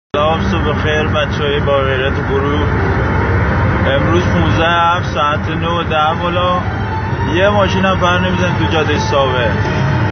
ساوه
یکشنبه ۱۵ مهر ۹۷ 🌐 ساوه

اعتصاب سراسری کامیونداران در اعتراض به گرانی وبی حقوقی معیشتشان